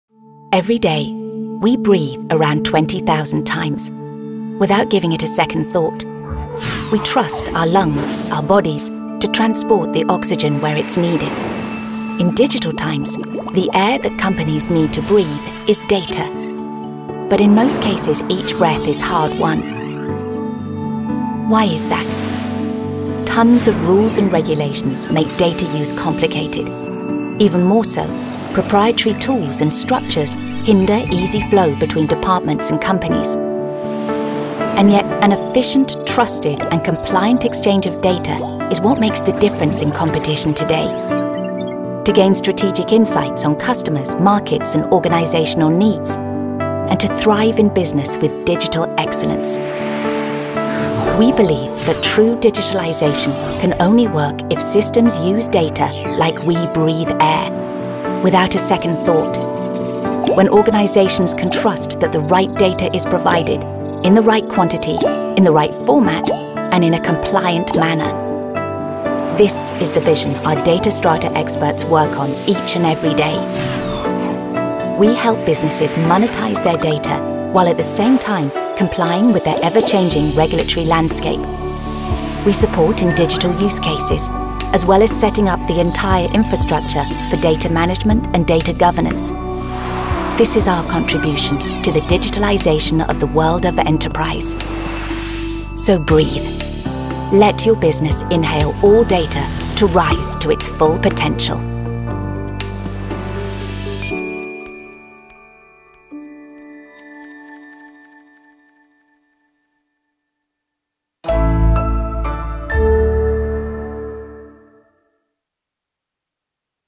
DataStrata by Deloitte | Unleash the full potential of data for a digitalized world
Every day, we breathe around twenty thousand times – without giving it a second thought.  We trust our lungs, our bodies, to transport the oxygen where it’s needed. In digital times, the air that companies need to breathe is data. Our DataStrata experts help organization to do exactly that to contribute to the digitalization of the world of enterprise.

Get more information about Deloitte on our website / Besucht auch unsere offizielle Website für News, aktuelle Studien, Trends, Stellenangebote und Infos rund um Deloitte: